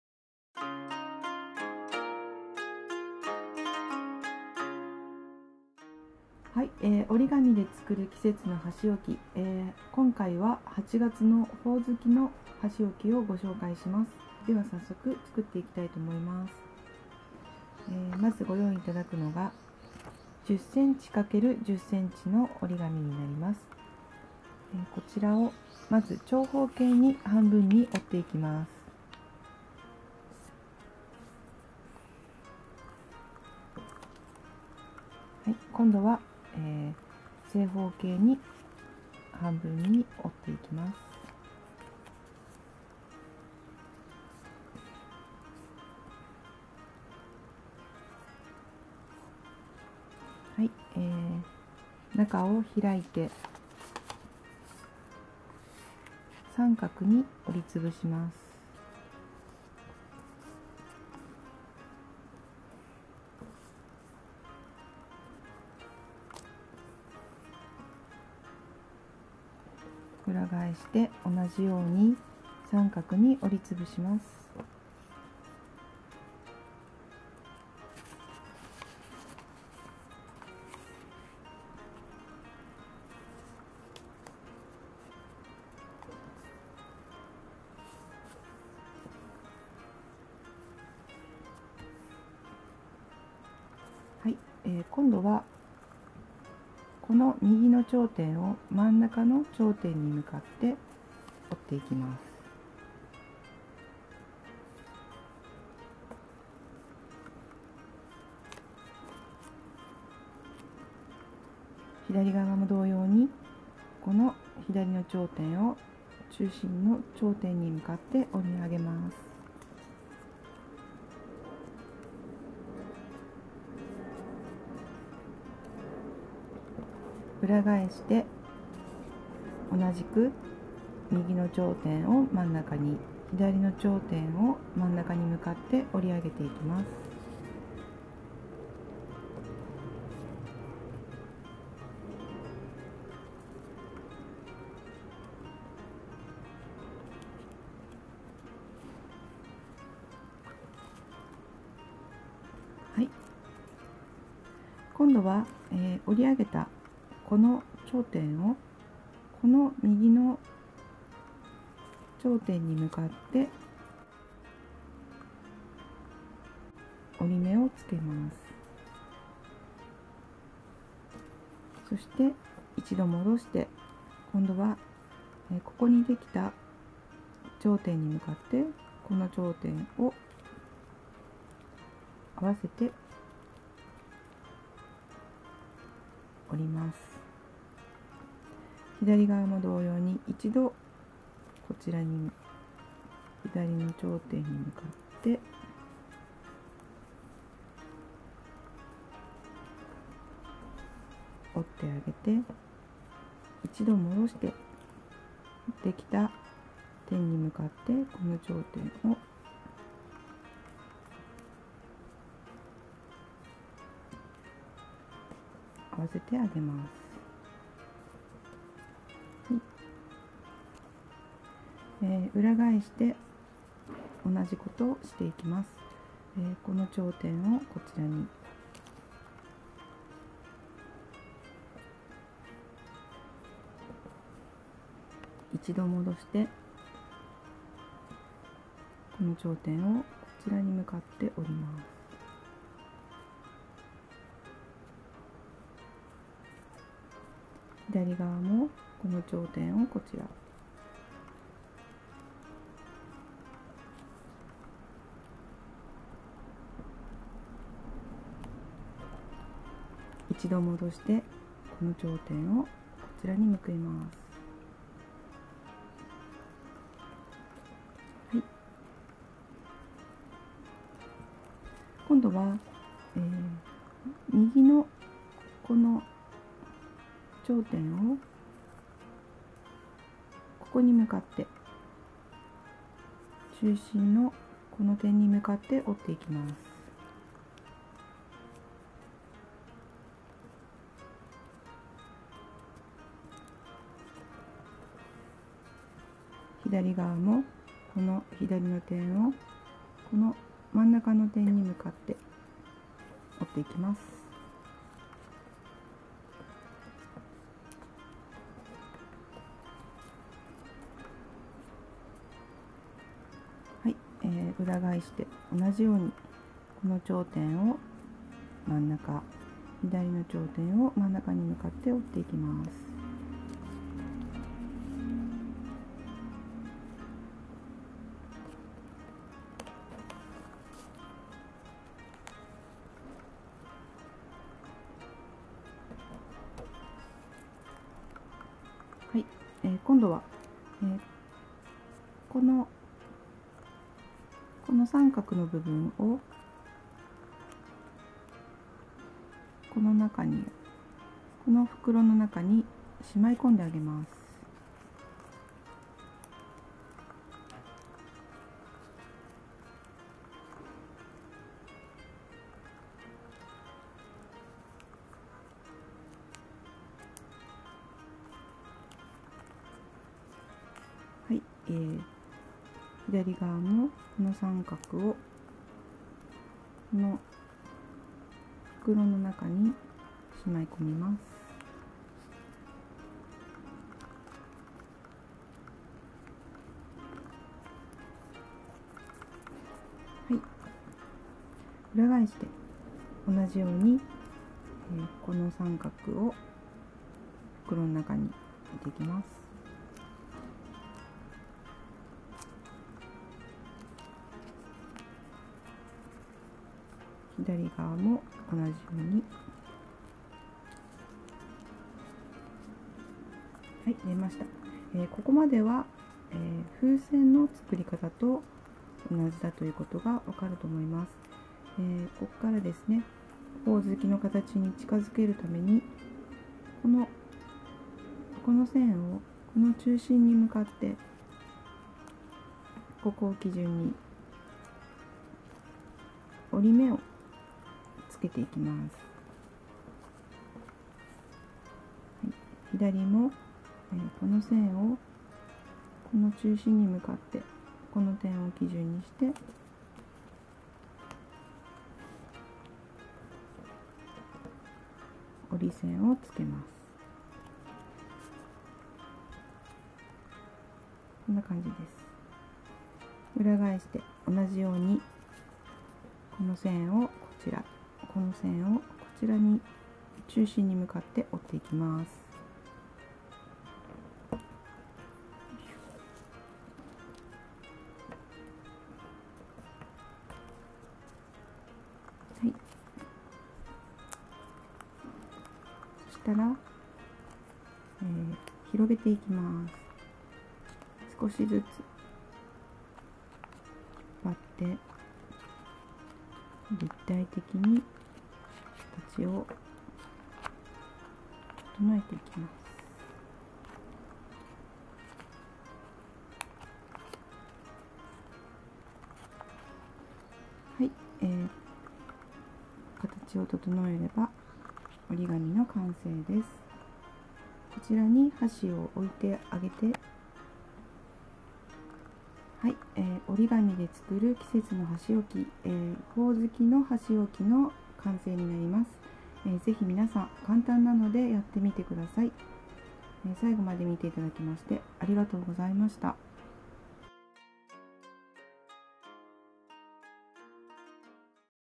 折り紙で作る箸置き~８月ほおづき~[How to ORIGAMI] Orijinal Winter cherry
８月のお盆の飾り付けによく使われるなど
夏の訪れを連想させるほおづきを,オリジナルバージョンでご紹介します♪

t is meant to be used in the decoration of Bon Festival in August in Japan.
I will introduce the original to "winter cherry" reminiscent of the coming of summer.

日本の折り紙の箸置きであなたの食卓を彩りませんか。
このチャンネルでは季節の箸置きの作り方を月ごとに­­ご紹介していきます。